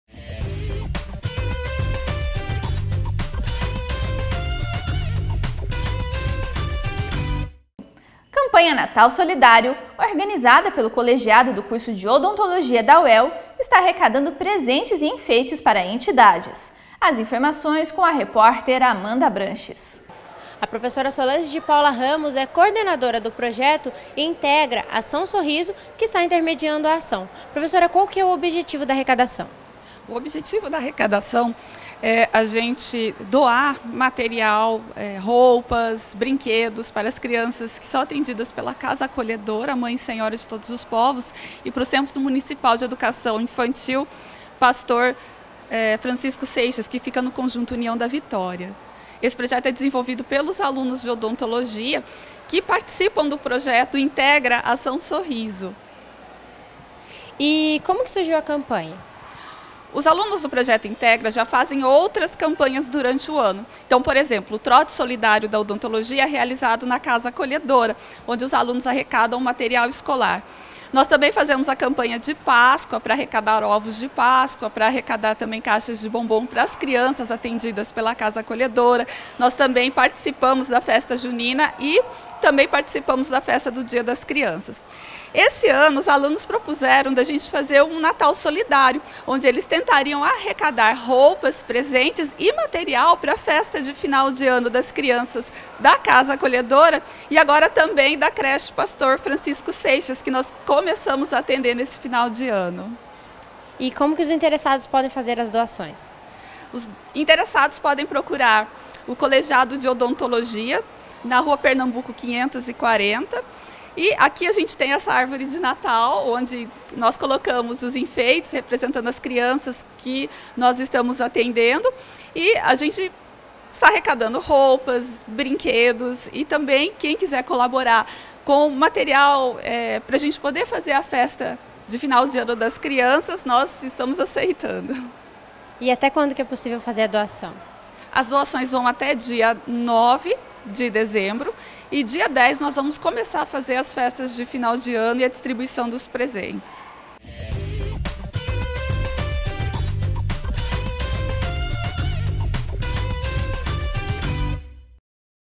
Colegiado de Odontologia organiza campanha "Natal Solidário"
A campanha está arrecadando presentes e enfeites que serão doados à Casa Acolhedora Mãe e Senhora de todos os povos e ao Centro Municipal de Educação Infantil Pastor Francisco Seixas. As doações podem ser feitas até o dia nove de dezembro.